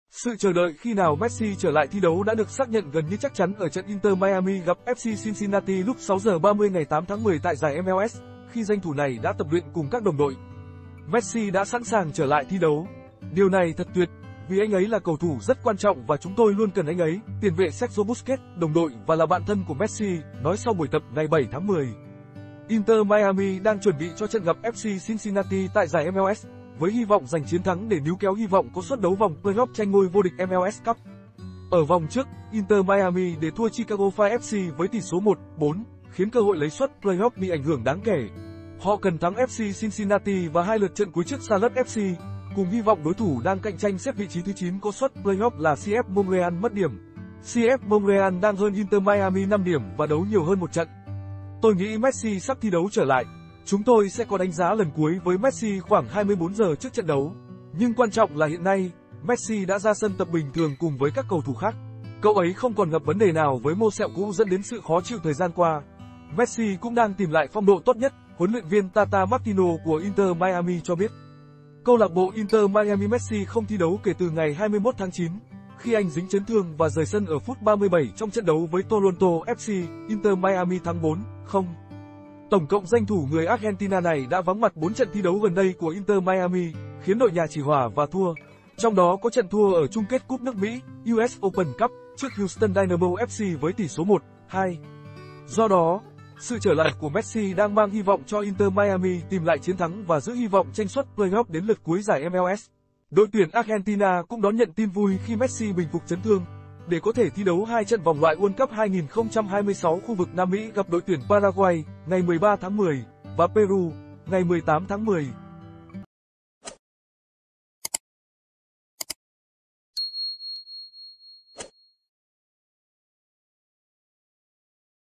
MESSI MANG TIN VUI CHO INTER MIAMI VÀ ĐỘI TUYỂN ARGENTINA
Sự chờ đợi khi nào Messi trở lại thi đấu đã được xác nhận gần như chắc chắn ở trận Inter Miami gặp FC Cincinnati lúc 6 giờ 30 ngày 8.10 tại giải MLS, khi danh thủ này đã tập luyện cùng các đồng đội.